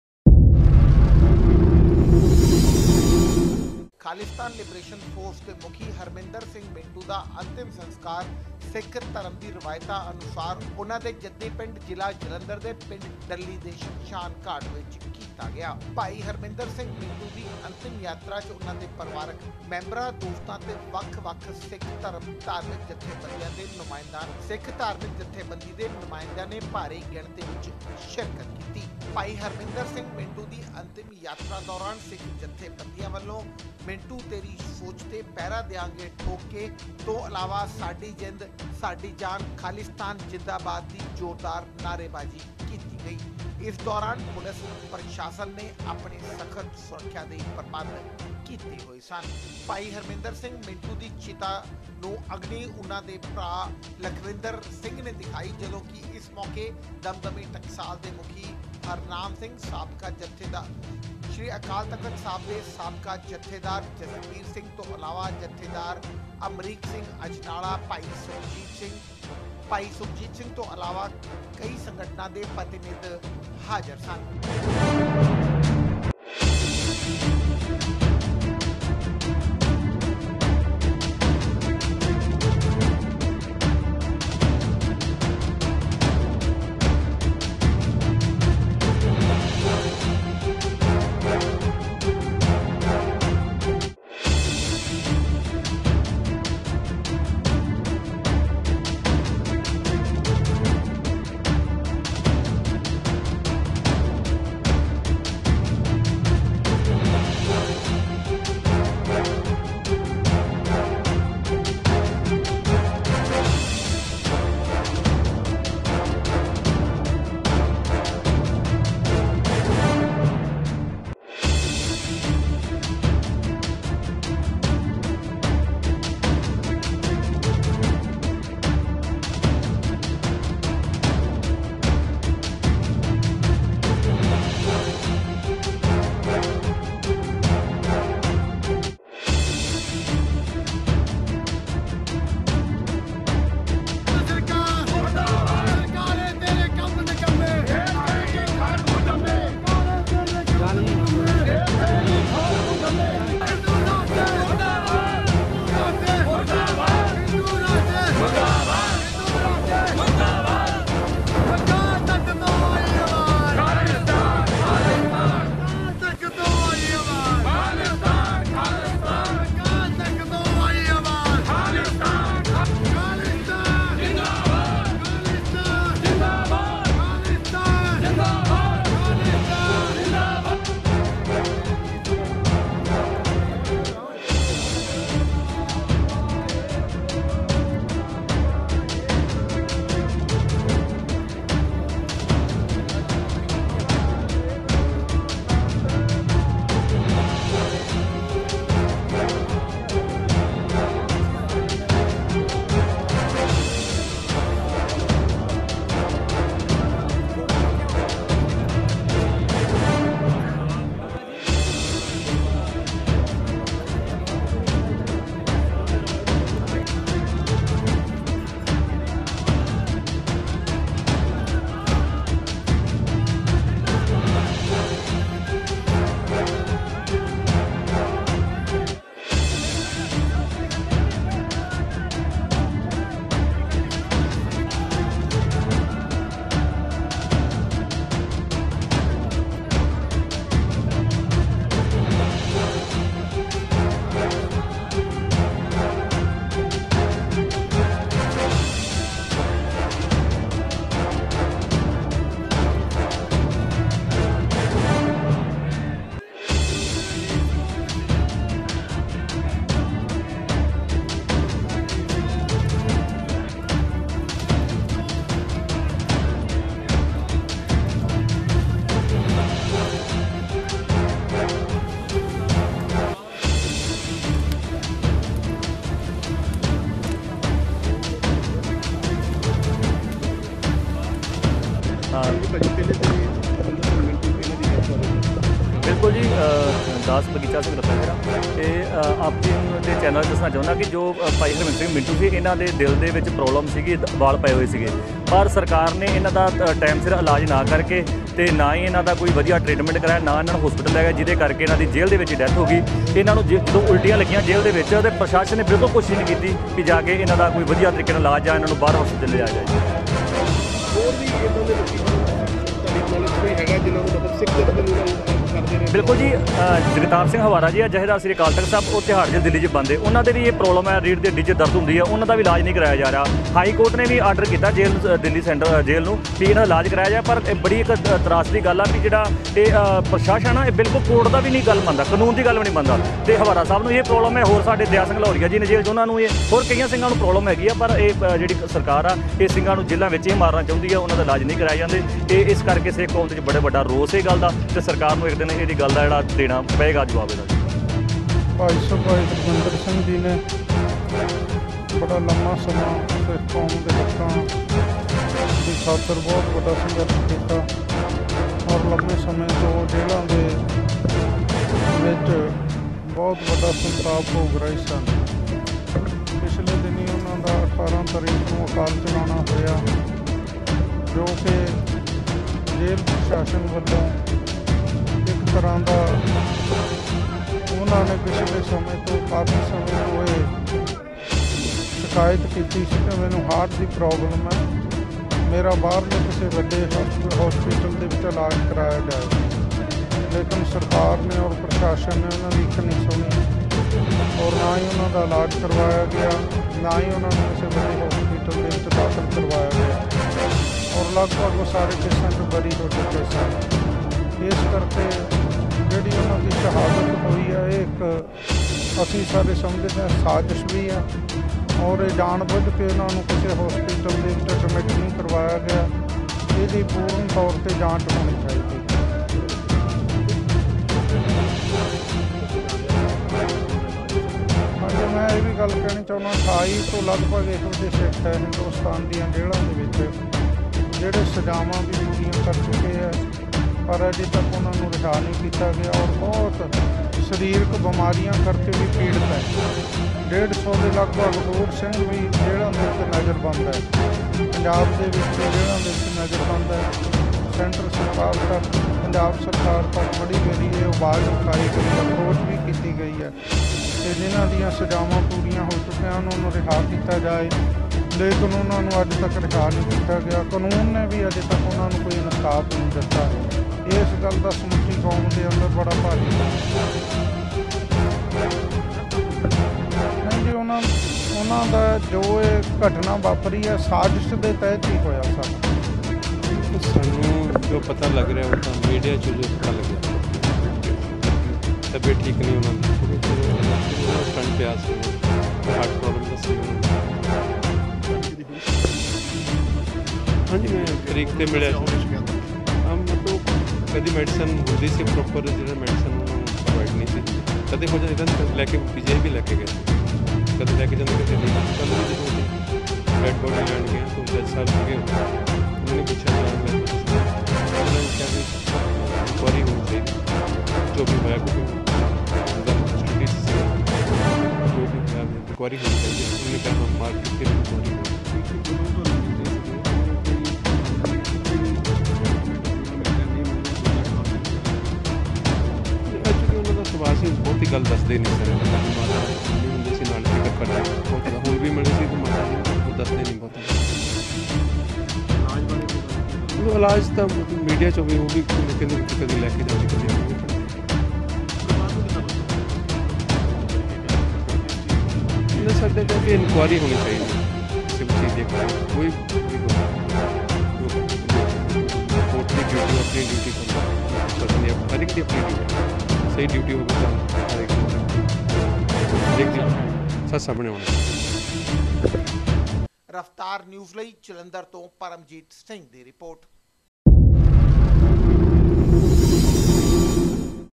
Bhai Harminder Singh Ji Mintoo Full Cremation with Sikhism
ਸਿੱਖ ਧਰਮ ਦੀਆਂ ਰਿਵਾਇਤਾਂ ਨਾਲ ਹੋਇਆ ਹਰਮਿੰਦਰ ਮਿੰਟੂ ਦਾ ਅੰਤਿਮ ਸੰਸਕਾਰ

ਖਾਲਿਸਤਾਨ ਲਿਬਰੇਸ਼ਨ ਫੋਰਸ ਦੇ ਮੁੱਖੀ ਹਰਮਿੰਦਰ ਸਿੰਘ ਮਿੰਟੂ ਦਾ ਅੰਤਿਮ ਸੰਸਕਾਰ ਸਿੱਖ ਧਰਮ ਦੀਆਂ ਰਿਵਾਇਤਾਂ ਅਨੁਸਾਰ ਅੱਜ ਉਨ੍ਹਾਂ ਦੇ ਜੱਦੀ ਪਿੰਡ ਡੱਲੀ ਦੇ ਸ਼ਮਸ਼ਾਨਘਾਟ ਵਿਖੇ ਕੀਤਾ ਗਿਆ। ਹਰਮਿੰਦਰ ਸਿੰਘ ਮਿੰਟੂ ਦੀ ਅੰਤਿਮ ਯਾਤਰਾ 'ਚ ਉਨ੍ਹਾਂ ਦੇ ਪਰਿਵਾਰਕ ਮੈਂਬਰਾਂ, ਮਿੱਤਰਾਂ ਤੇ ਵੱਖ-ਵੱਖ ਸਿੱਖ ਧਾਰਮਿਕ ਜੱਥੇਬੰਦੀਆਂ ਦੇ ਨੁਮਾਇੰਦੇ ਭਾਰੀ ਗਿਣਤੀ 'ਚ ਸ਼ਾਮਲ ਹੋਏ। ਅੰਤਿਮ ਯਾਤਰਾ ਦੌਰਾਨ ਸਿੱਖ ਜੱਥੇਬੰਦੀਆਂ ਵਲੋਂ ''ਮਿੰਟੂ ਤੇਰੀ ਸੋਚ ਤੇ ਪਹਿਰਾ ਦਿਆਂਗੇ ਠੋਕ ਕੇ'', '' ਸਾਡੀ ਜ਼ਿੰਦ ਸਾਡੀ ਜਾਨ ਖਾਲਿਸਤਾਨ ਜ਼ਿੰਦਾਬਾਦ'' ਜ਼ੋਰਦਾਰ ਨਾਅਰੇਬਾਜੀ ਕੀਤੀ ਗਈ। ਇਸ ਦੌਰਾਨ ਪੁਲਸ ਪ੍ਰਸ਼ਾਸਨ ਨੇ ਸਖਤ ਸੁਰੱਖਿਆ ਦੇ ਪ੍ਰਬੰਧ ਕੀਤੇ ਹੋਏ ਸਨ।